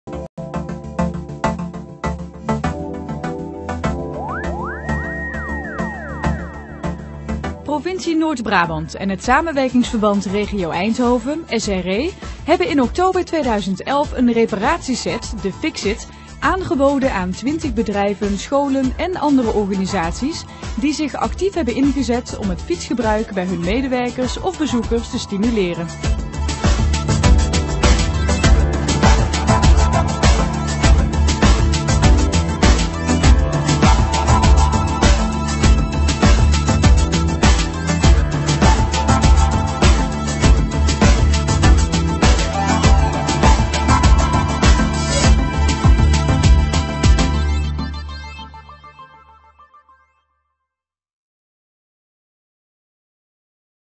Fix it: stimuleren fietsgebruik
De provincie en het Samenwerkingsverband Regio Eindhoven (SRE) belonen twintig bedrijven, scholen en organisaties die zich actief hebben ingezet om het fietsgebruik bij hun medewerkers of bezoekers te stimuleren. Deze organisaties krijgen van de provincie of het SRE een FixIt aangeboden."